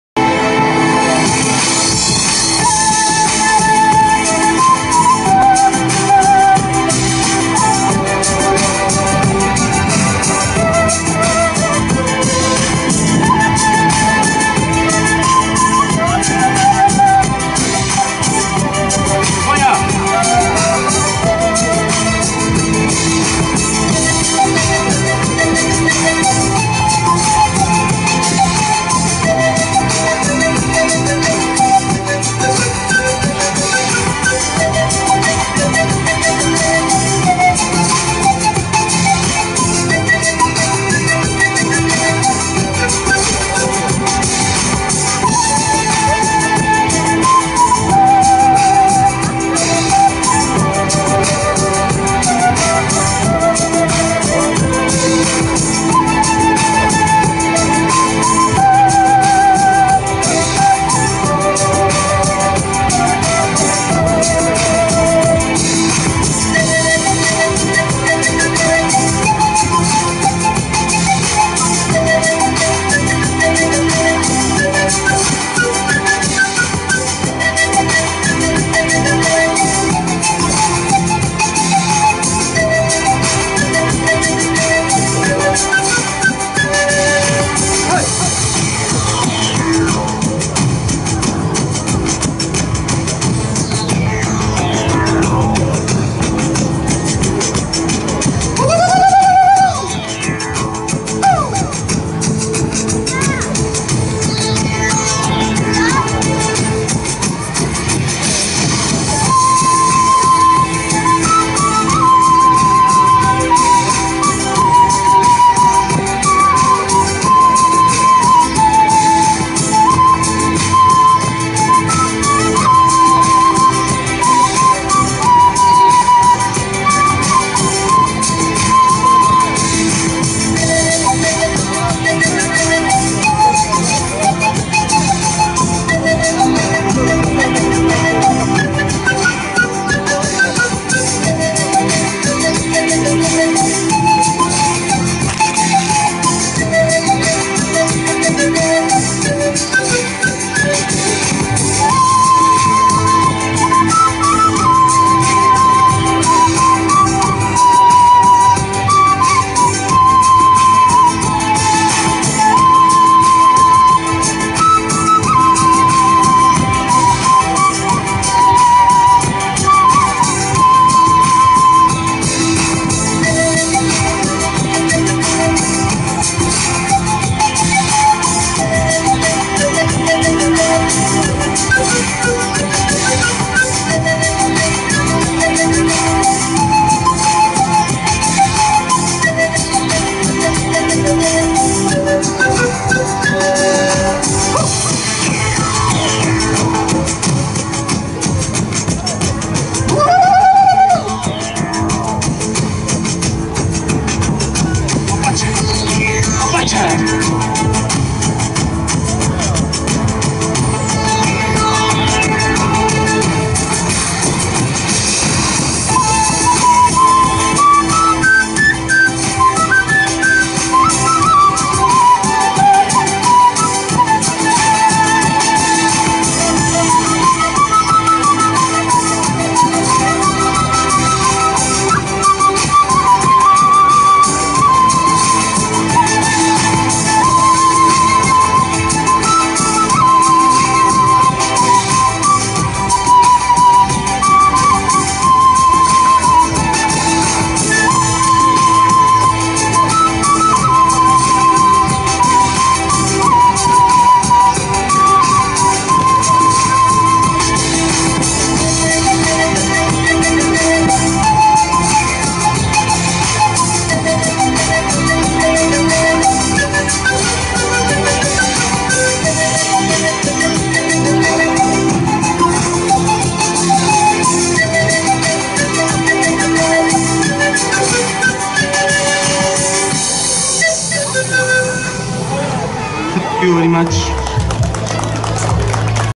페루전통악기 펜플릇연주와 춤
설명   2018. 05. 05.  복사골 제34회 예술제 축하공연